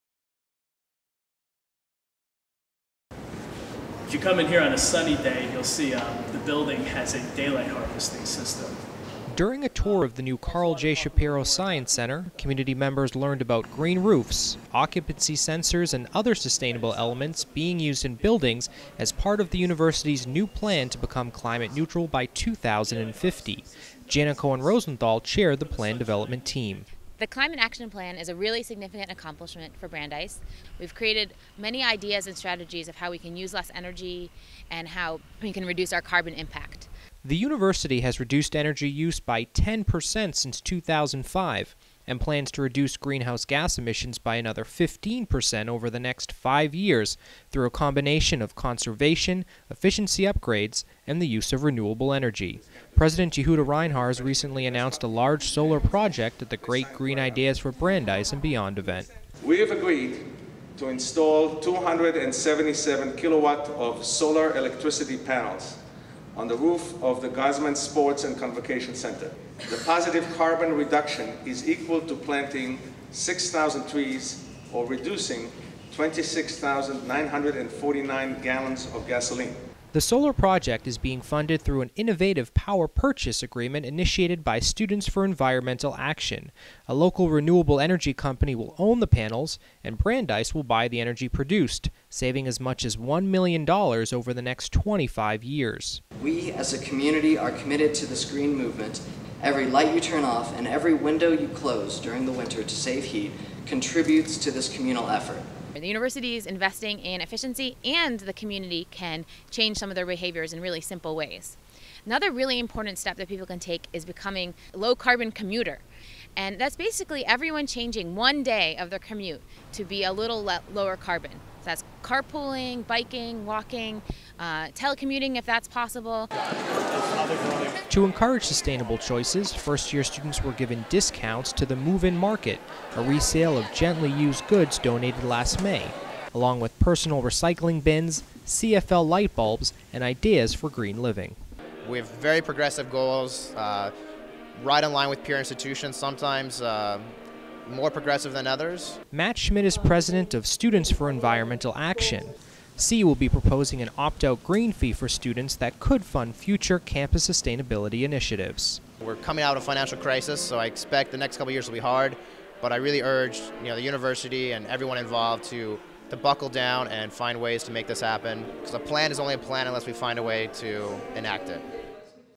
Brandeis striving to become climate neutral by 2050
Brandeis has developed a Climate Action Plan, which outlines strategies to reduce campus greenhouse gas emissions and become climate neutral by 2050.